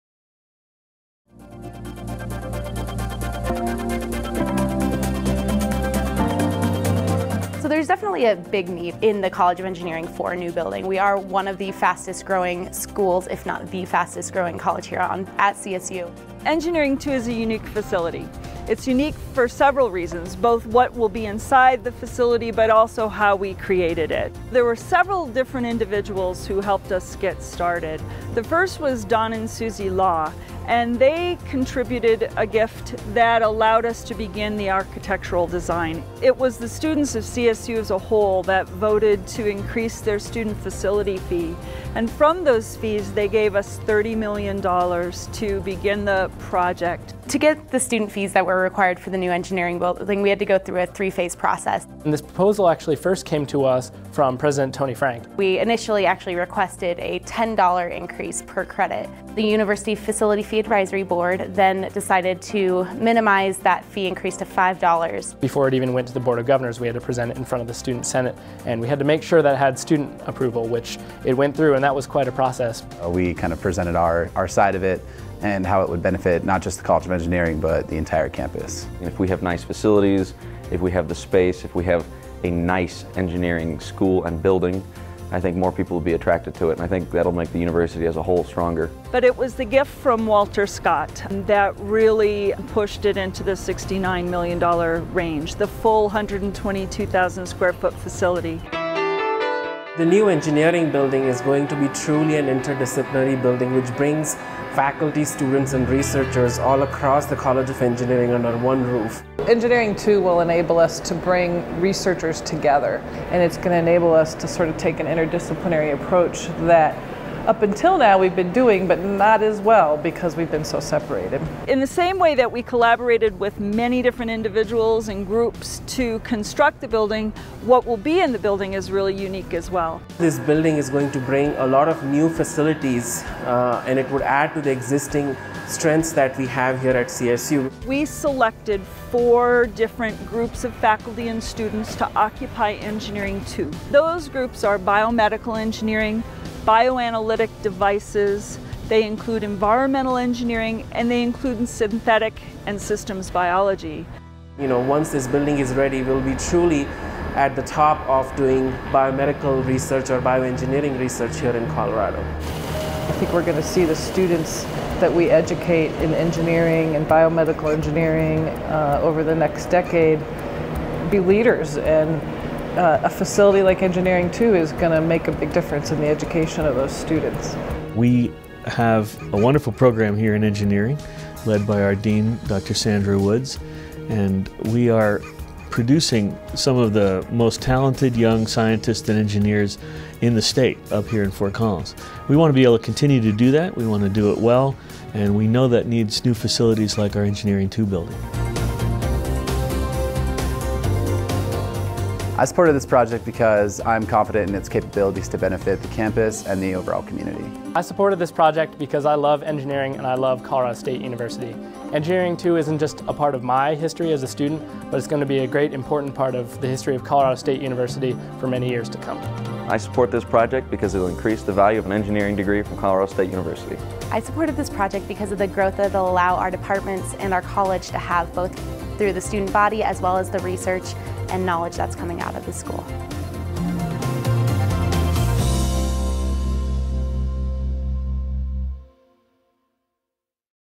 Colorado State University Starts Construction on Engineering II
Level 3 Communications Chairman Walter Scott Jr., a Colorado State alumnus, and his wife, Suzanne, have committed $10 million to a new 122,000-square-foot engineering building at Colorado State. Listen to students and faculty talk about the project and the generosity of people like the Scotts and students who voted themselves increased facility fees for this unique learning environment.